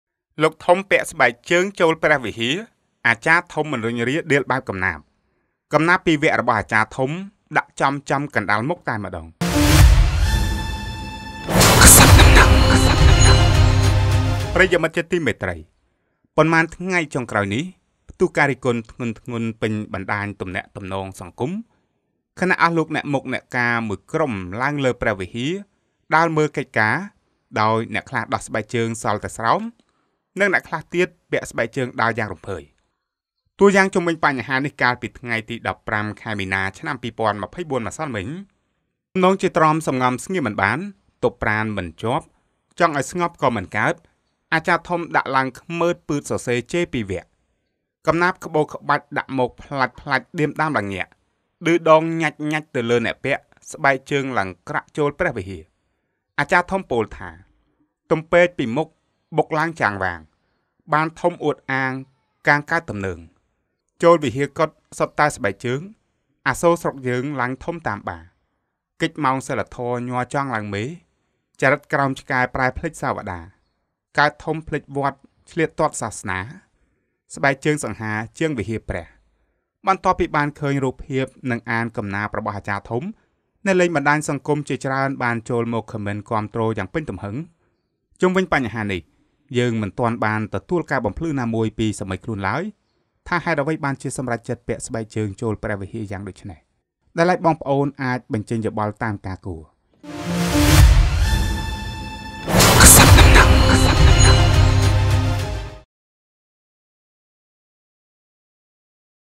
លោកធំពាក់ស្បែកជើងចូលព្រះវិហារ អាចារ្យធំមិនរួញរា.....បែបកំណាព្យ.QCN, Khmer news.
លោកធំពាក់ស្បែកជើងចូលព្រះវិហារ អាចារ្យធំមិនរួញរា. បែបកំណាព្យ។
ទាក់ទងផ្សាយពាណិជ្ជកម្ម៖ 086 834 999/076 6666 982